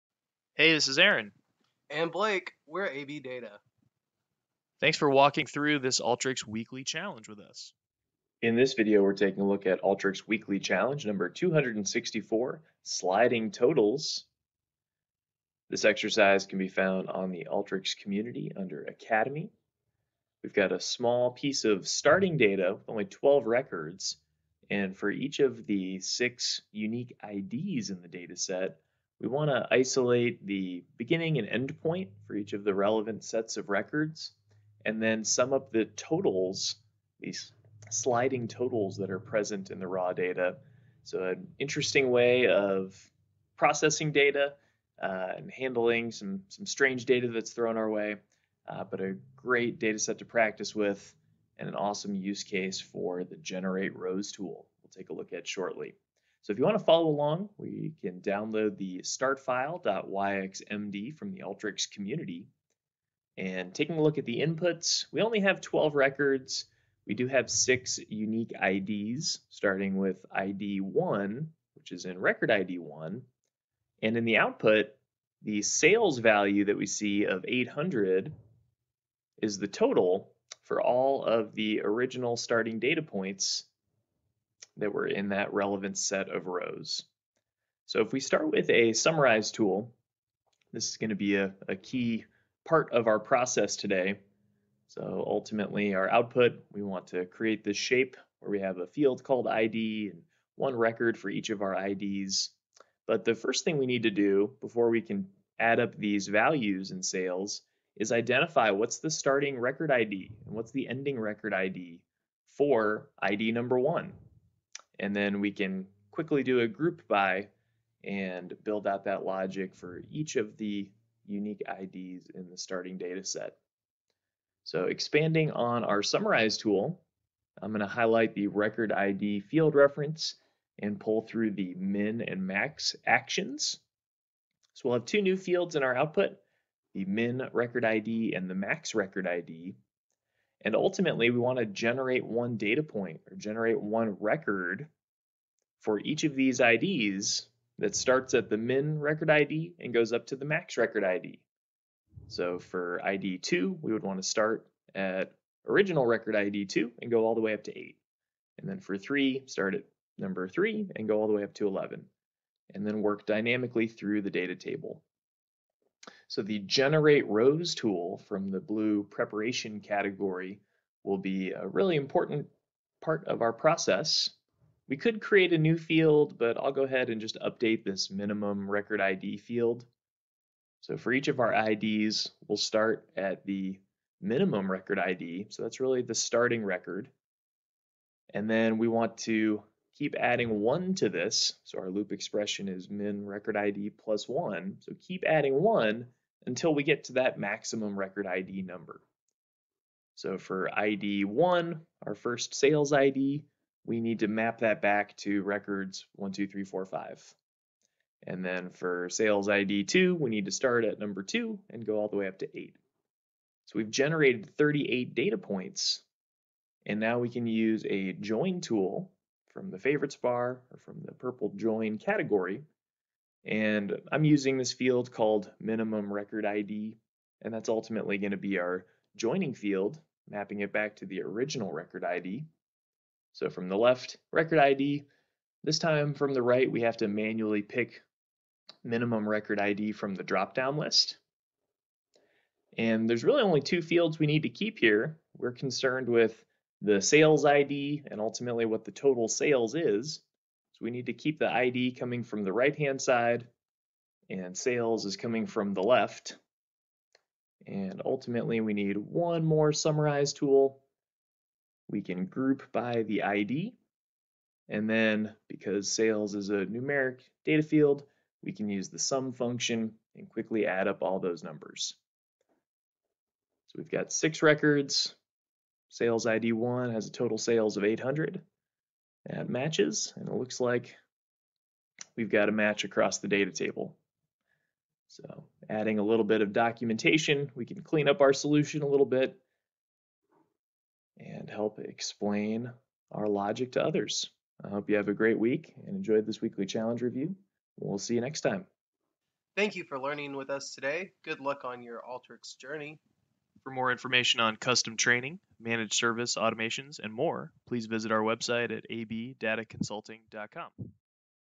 Alteryx Weekly Challenge #264 - AB Data Consulting
Join us while we build our solution for Alteryx Weekly Challenge #264!

Everyone loves a good invoice! There’s just something about seeing the amount you paid for each item and finding that everything is as it should be, adding up to the total amount. And for the most part invoices are created by automated processes which reduce the chance of errors appearing (unless someone entered the wrong items). But what if something went wrong…

For this week’s challenge, pretend you are a business analyst for a service provider which offers a number of services. You are attempting to report the services delivered for last month. Due to data limitations of your database the data comes in a unique way which you need to calculate the rolling sales in order to work out the total of services provided. The total sales are all the sales between (and including) each matching pair of IDs. In other words, you need to find the total of all of the sales for the records beginning with a given ID through to the corresponding ID, making sure to include all records between your pair of matching IDs.